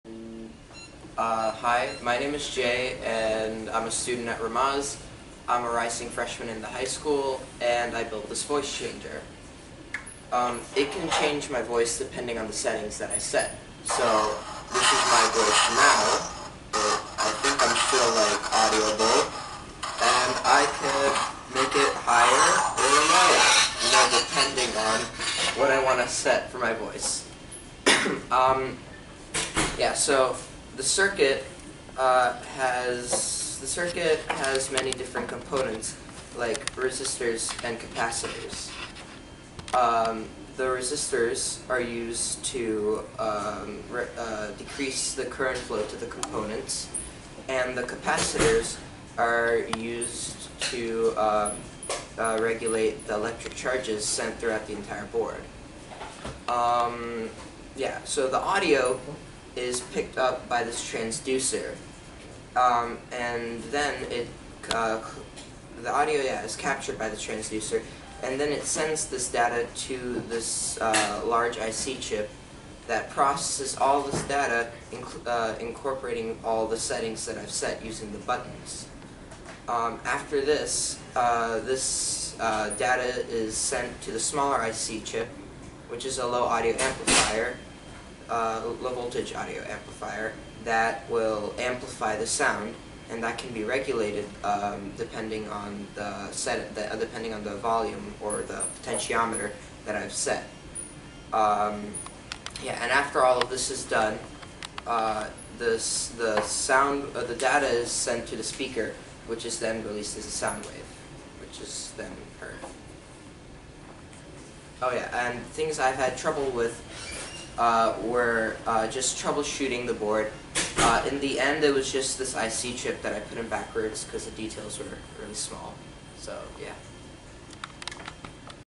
Jay Starter Project - Voice Changer
NYC starts off the summer right with a completed voice changer! Great Job Jay!